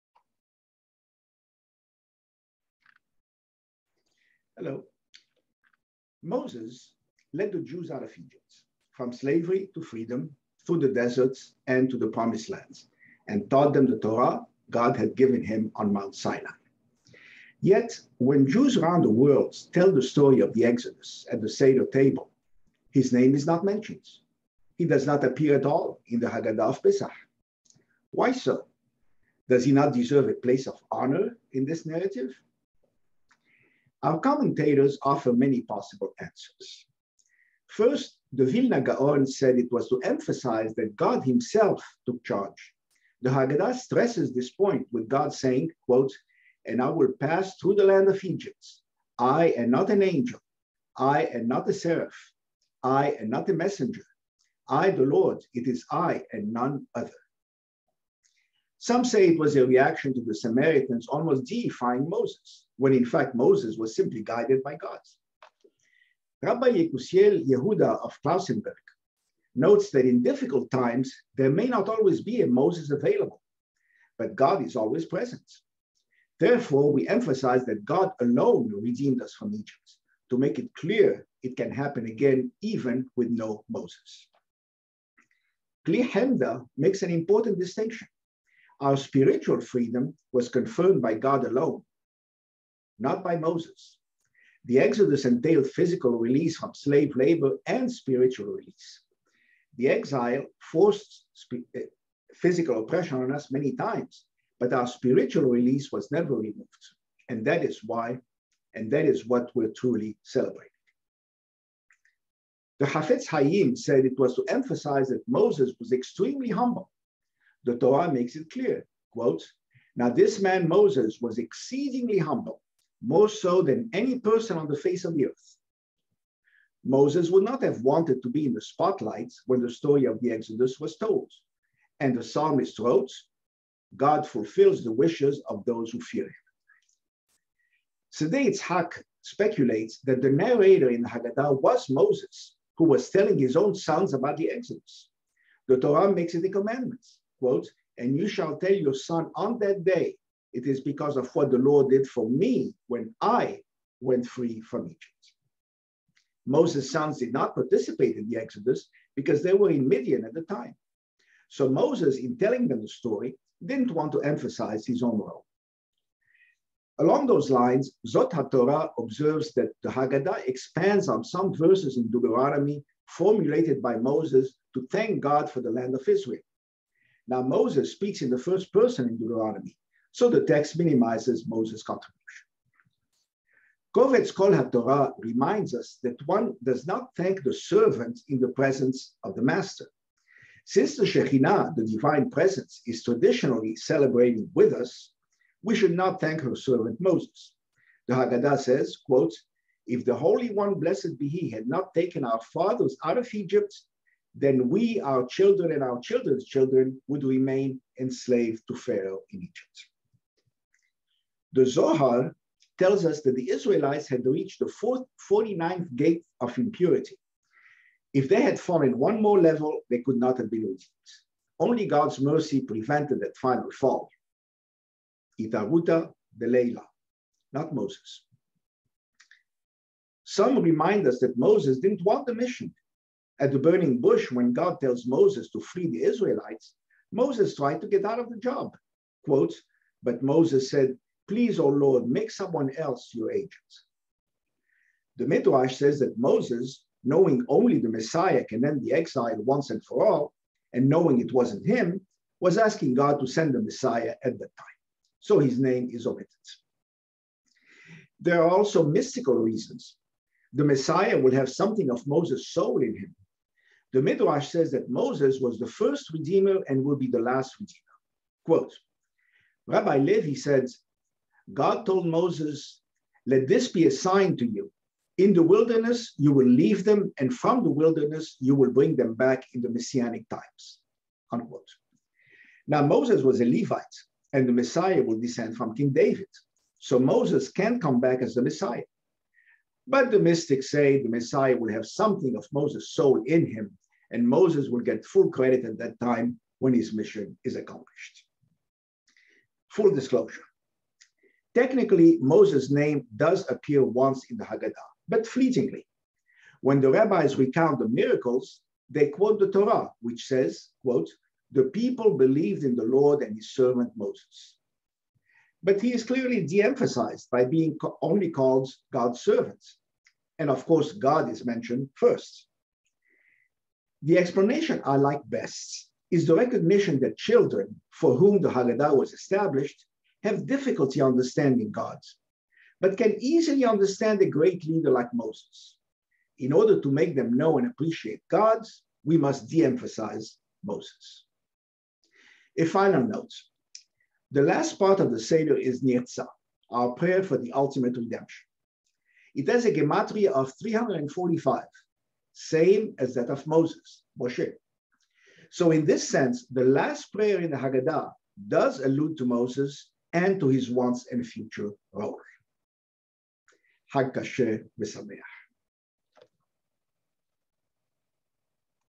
Dr Maurice Mizrahi - Why is Moses not in the Haggadah? (Pessah)
When Jews around the world tell the story of the Exodus at the seder table on Pessah, Moses' name is not mentioned.  But did he not lead the Jews out of Egypt, from slavery to freedom, through the desert and to the Promised Land, and taught them the Torah God had given him on Mount Sinai? Does he not deserve a place of honor in this narrative?  Commentators have given many reasons why Moses is omitted from the story.  Let us explore them together.

Volume 1
Volume 2
Volume 3
Volume 4
Volume 1 in French
Volume 2 in French
Volume 3 in French
Volume 1 in Italian
Volume 2 in Italian